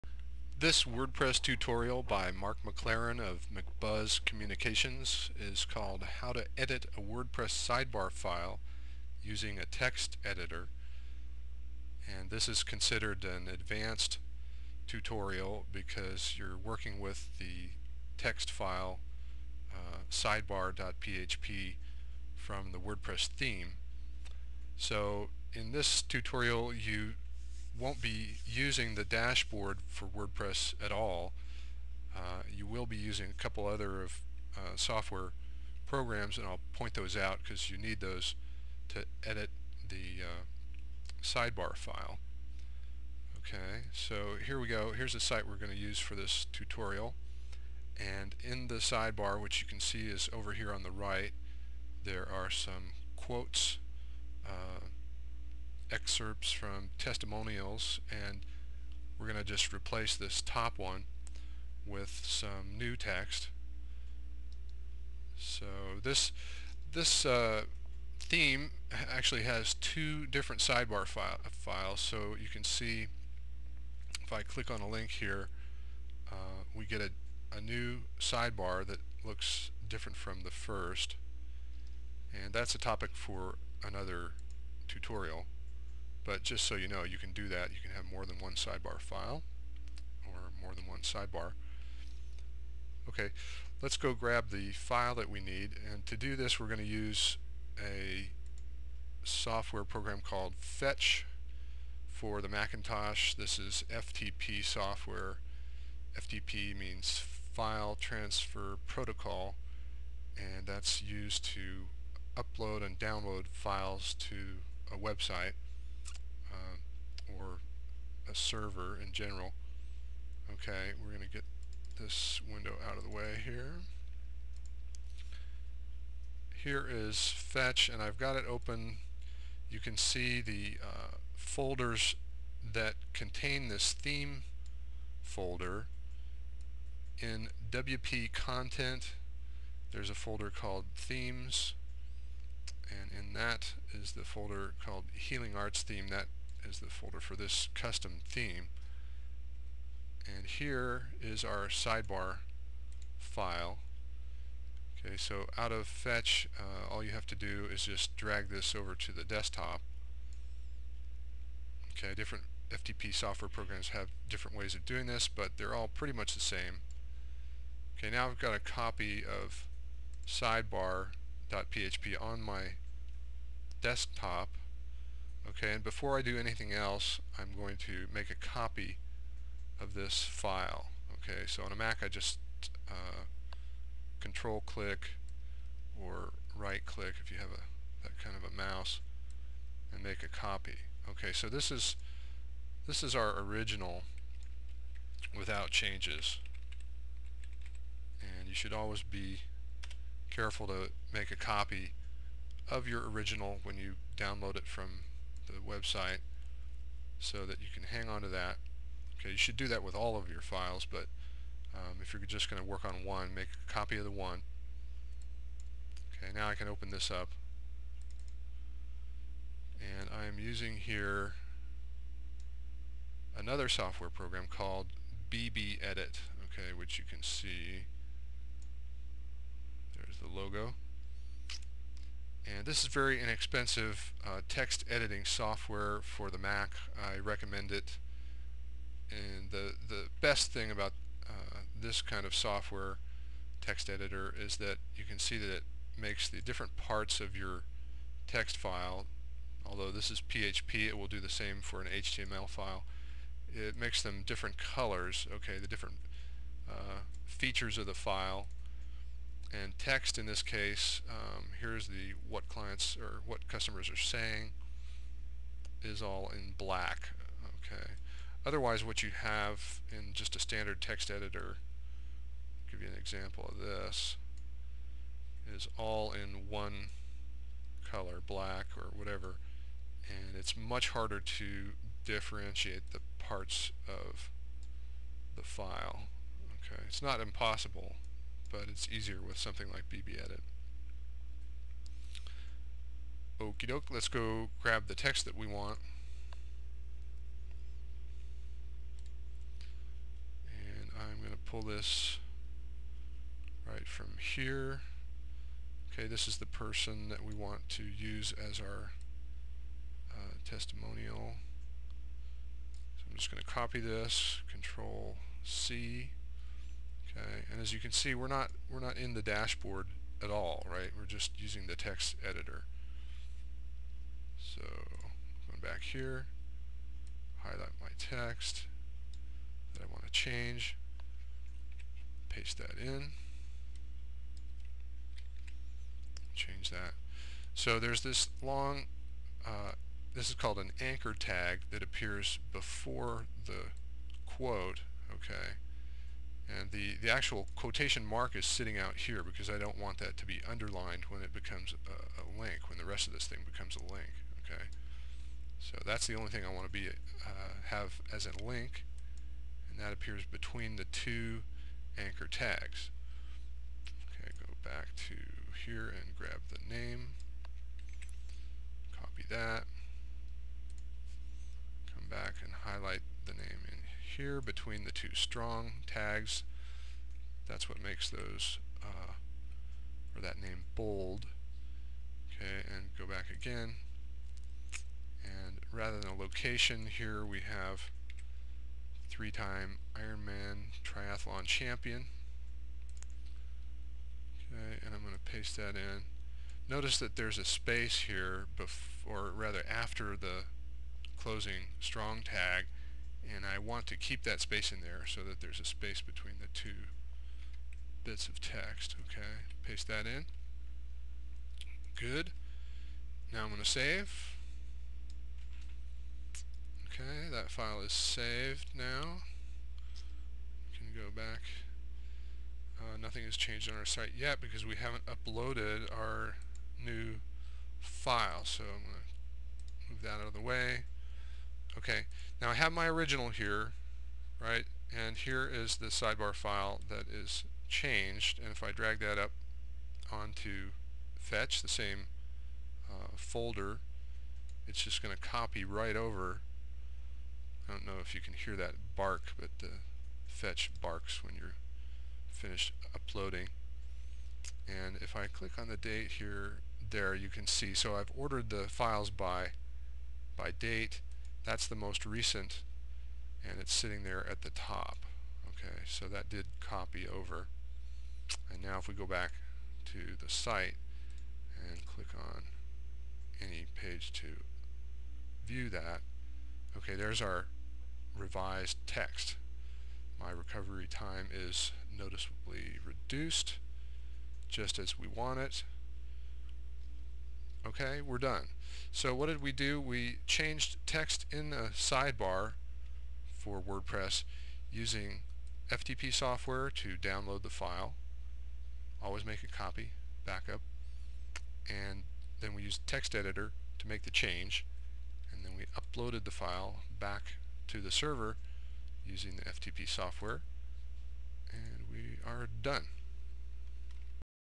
WordPress Tutorial - Edit a Sidebar File Using a Text Editor
This Advanced-level WordPress tutorial shows how to download, edit and upload a WordPress theme sidebar file (sidebar.php) using FTP software and a text editor. This is considered an advanced WordPress tutorial because you work with the files for the WordPress theme. You do not use the WordPress Dashboard in this tutorial. The tutorial assumes you know how to setup and use FTP software on your computer so that you can download and upload the sidebar file. It also assumes you know how to open a text file like sidebar.php using a text editor. On a PC, common text editors are Notepad and Wordpad. On a Mac, the default text editor is TextEdit. A good Macintosh text editor for HTML and PHP files is BBEdit.